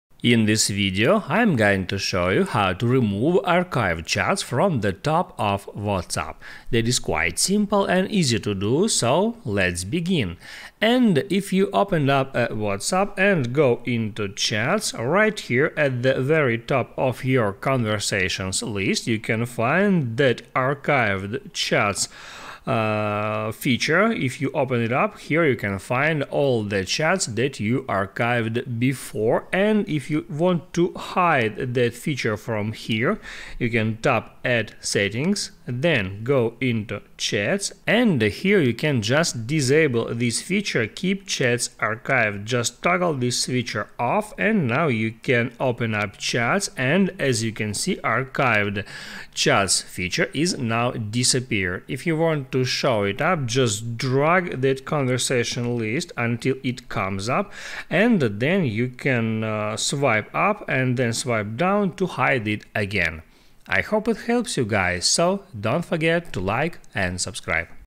Remove Archived Chats From the Top of Whatsapp
In this video i show simple way Remove Archived Chats From the Top of Whatapp